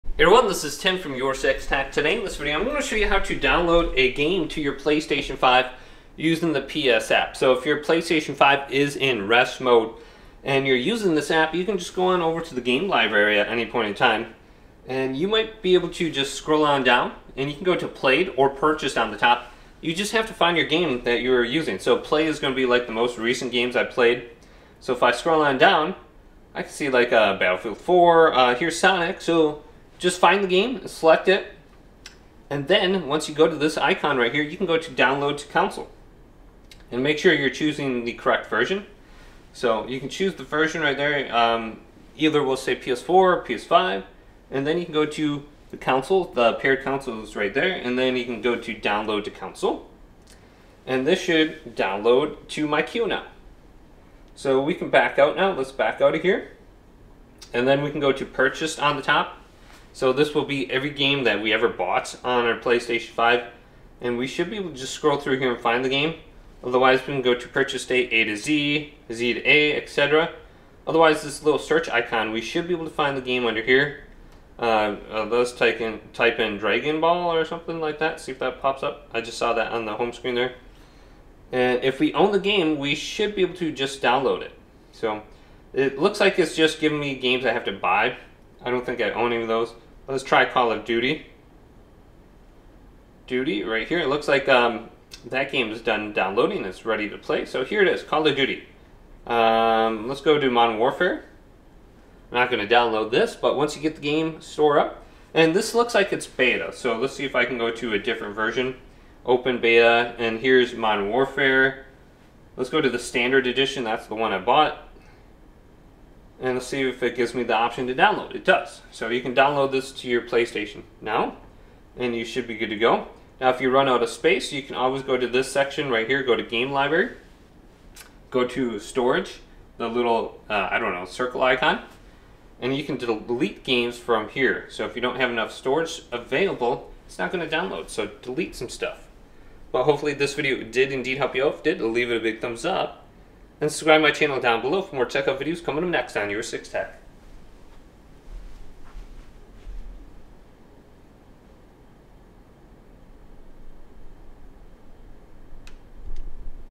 How to Download a Game to your PS5 from the PS App on Phone (Settings Tutorial)
To download a game to your PS5 using the PlayStation app on your phone, follow these steps:

Link Your PS5 to the PlayStation App:
Ensure that your PS5 console is linked to the PlayStation app on your iPhone, iPad, or Android device.
Open the app and navigate to the settings menu (tap the gear icon on the app’s home screen).
Scroll down to “Console Management” and select “Link Console to App.”
Choose your PS5 from the list of consoles displayed (make sure you’re signed in to the same PlayStation account on both the console and the app).
Prepare Your PS5:
On your PS5, go to the Settings menu (gear icon on the home screen).
Scroll down to “System” and select “Power Saving.”
From the submenu, choose “Features Available in Rest Mode.”
This allows your PS5 to update automatically while in standby mode.
Optionally, enable “Enable Turning On PS5 From Network” to turn on your PS5 using the app.
Download Games From the App:
Open the PlayStation mobile app on your smartphone or tablet.
Access your game library (second tab from the right at the bottom of the app).
Select the game you want to download.
If it’s not in the list of recently played games, switch to the “Purchased” list (which sorts your library alphabetically).
Tap the “Download to Console” option below the selected game.
The download progress will appear in the queue.
You’ll receive a notification when the game is ready to play on your PS5.